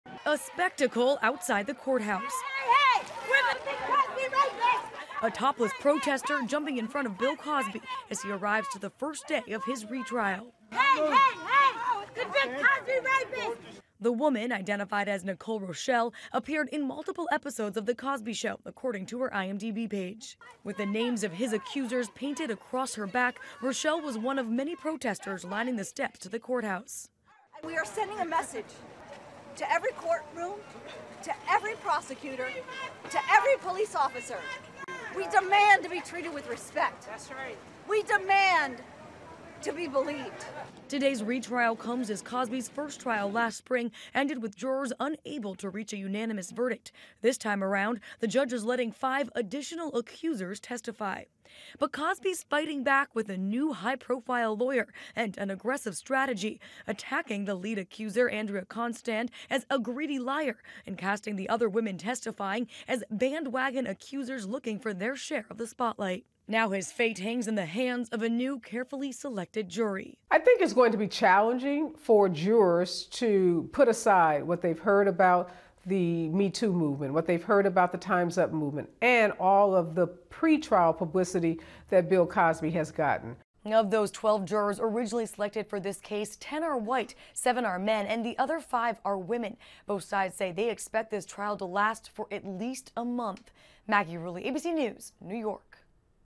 Topless protester who allegedly lunged at Bill Cosby ID'd as actress on his hit show
Cosby's retrial got off to wild star when topless woman allegedly charged him.

Bill Cosby paid accuser $3.38 million to keep quiet about sexual assault: Prosecutor

The retrial of Bill Cosby got off to a wild start Monday when a topless woman who once acted on "The Cosby Show" charged at the comedian as he arrived at a Pennsylvania courthouse, but once the case began it was a prosecutor who laid bare a long-held secret.

For first time ever, the prosecution publicly said the 80-year-old Cosby paid his primary accuser $3.38 million in exchange for her silence about him allegedly drugging and sexually assaulted her.

Montgomery County District Attorney Kevin Steele described the story of Cosby and accuser Andrea Constand as a "he said, she said" scenario and spent much of his opening argument blunting anticipated defense attacks on Constand, the prosecution's chief witness.